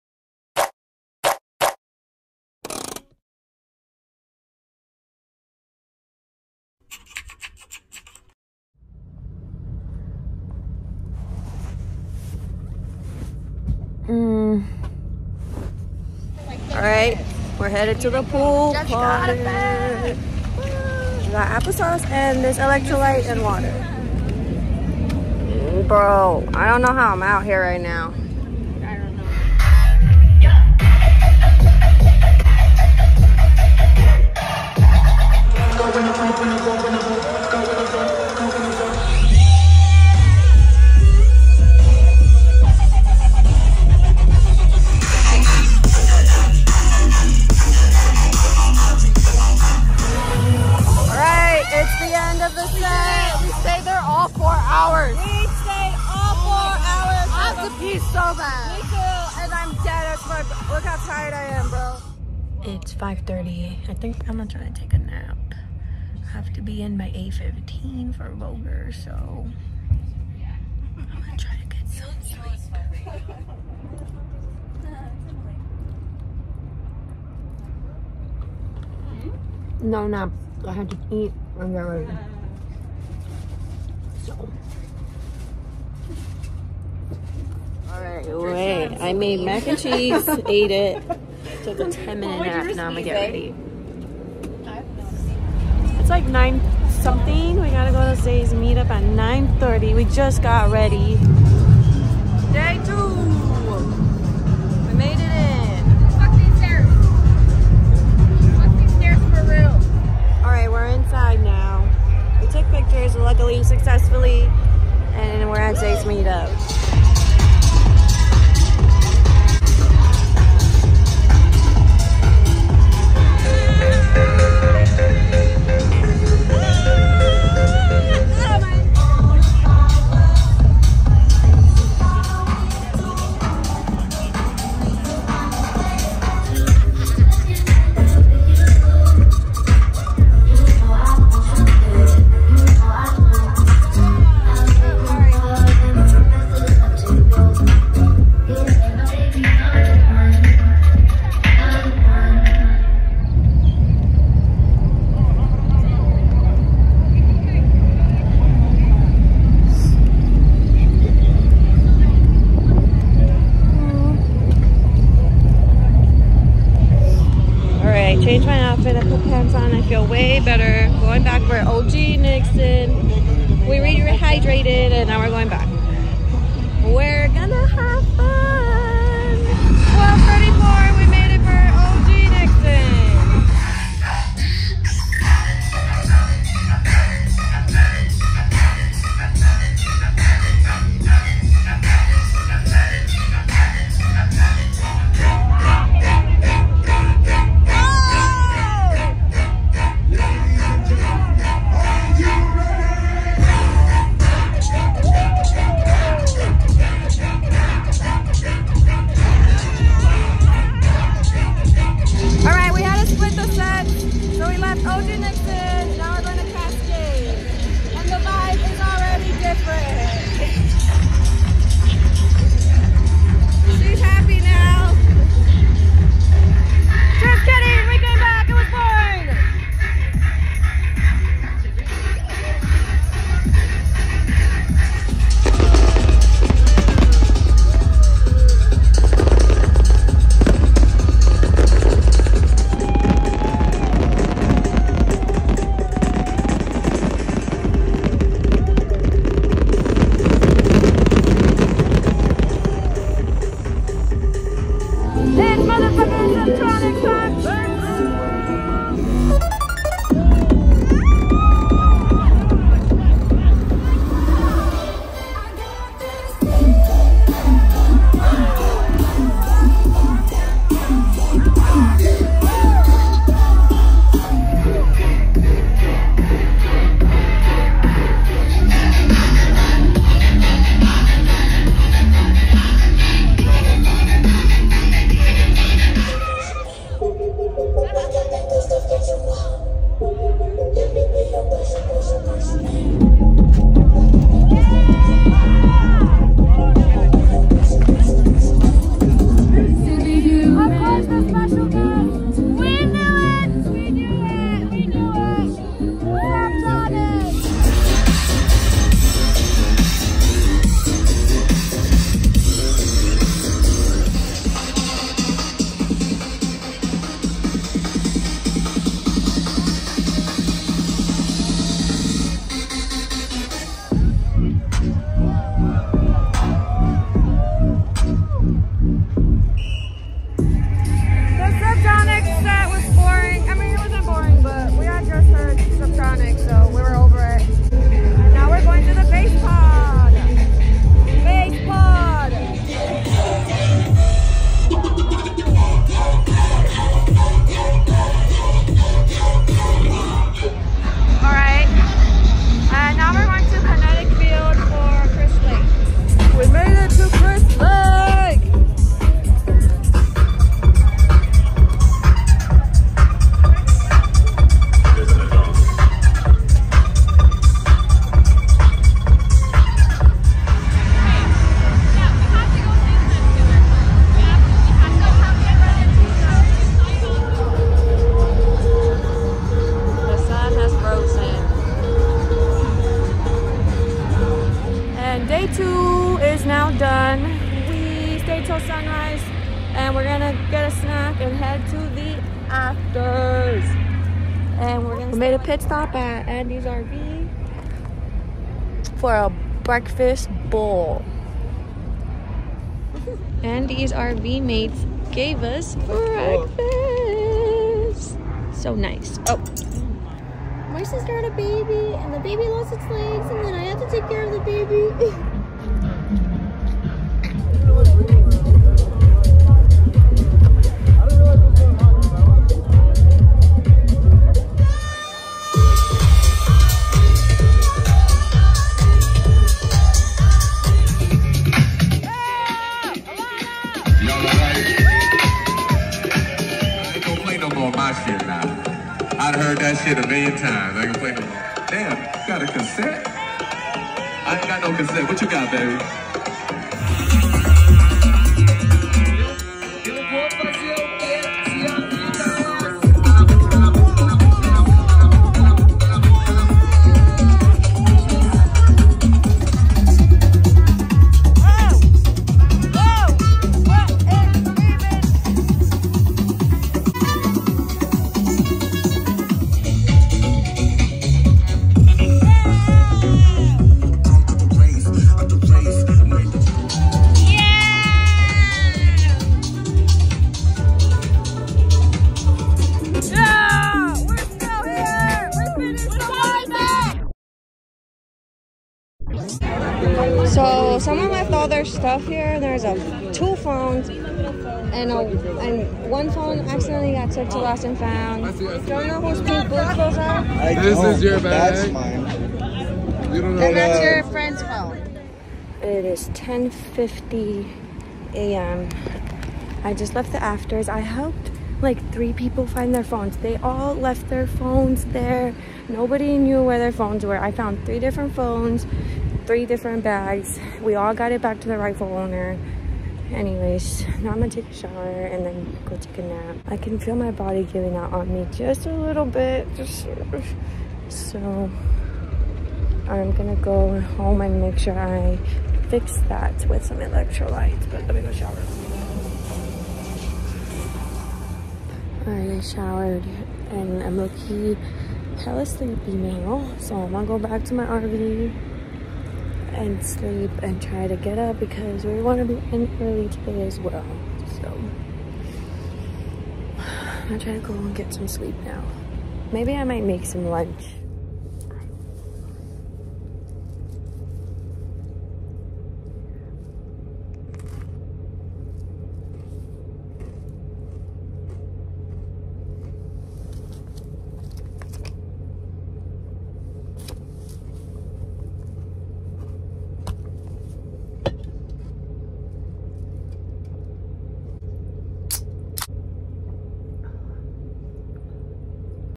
EDC LAS VEGAS | DAY TWO 💚
Day two the gang went as teletubbies :) I woke up and ran off two hours but made it to the full day of pool partying.